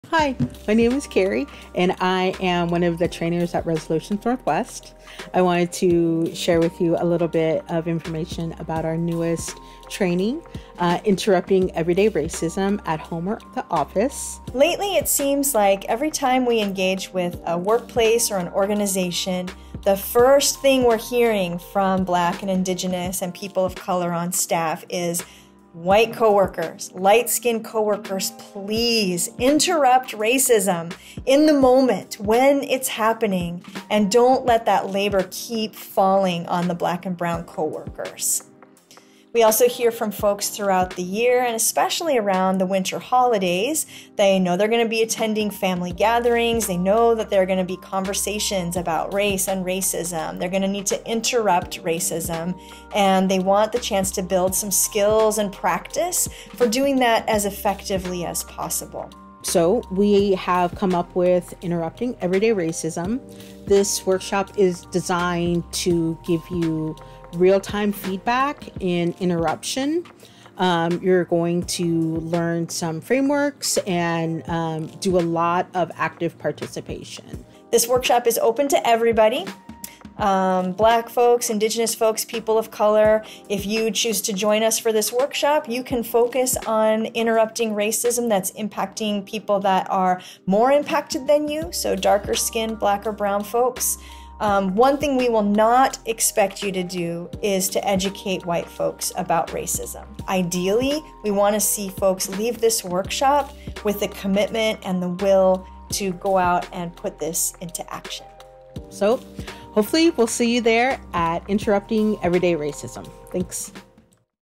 Interrupting Everyday Racism - Family & Workplace | Training & Consultation
Designed as a learning space for white and light skin folks who notice a gap in how they WANT to show up for anti-racist work, and what they actually DO.

This space is open to Black, Indigenous, and people of color, with a focus on interrupting racism directed to people more impacted than you: Black and darker skin folks. BIPOC folks will NOT be expected to teach white folks.